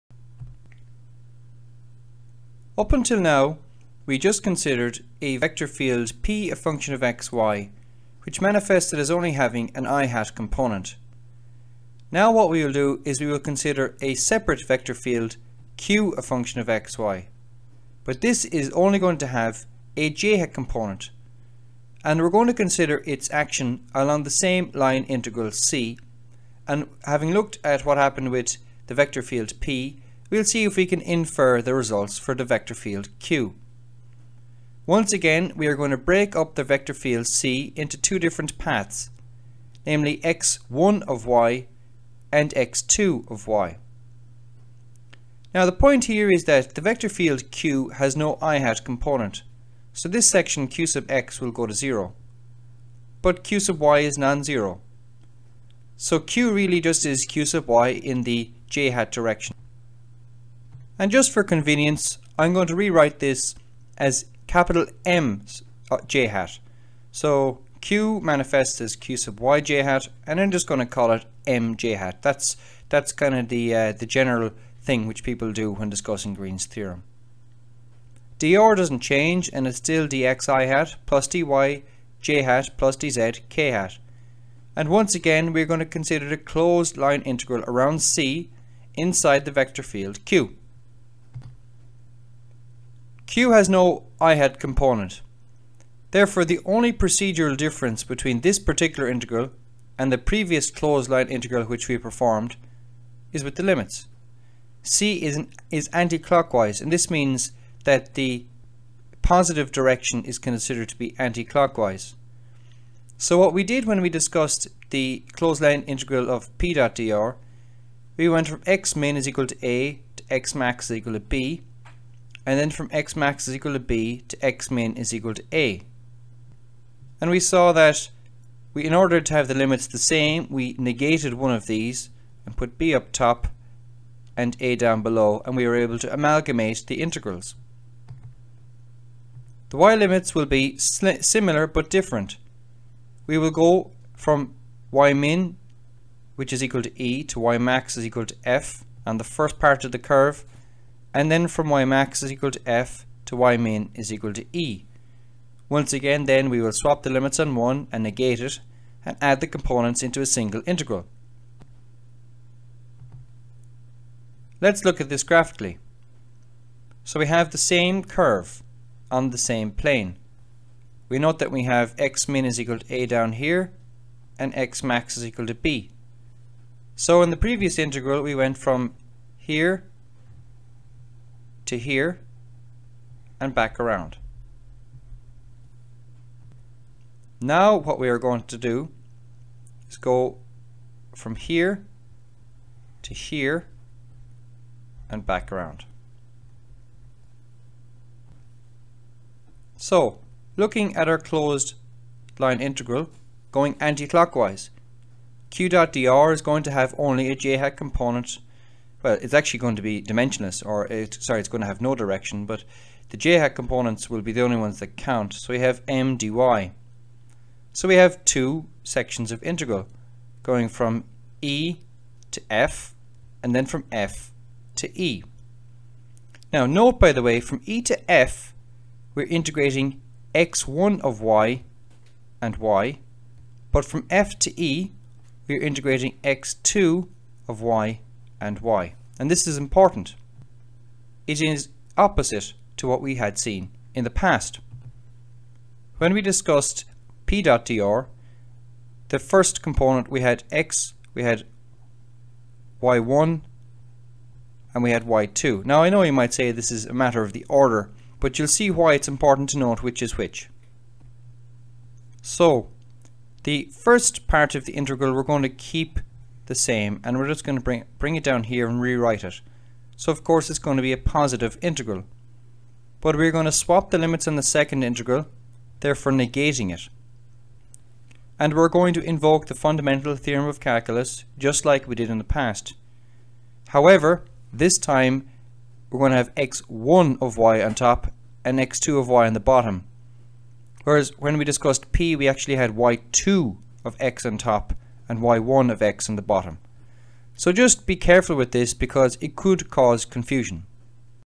Green's Theorem Derivation #1 4/5: Closed Line Integral j-hat Field
Learn all about Green's Theorem from two different derivations of same. Here's part #4/5 of derivation 1/2.

This video is part of a Complex Analysis series where I derive the Planck Integral which is required in order to study Blackbody radiation.

This is quite a difficult integral to solve and it requires complex analysis. Each video in this series helps to build the skills required.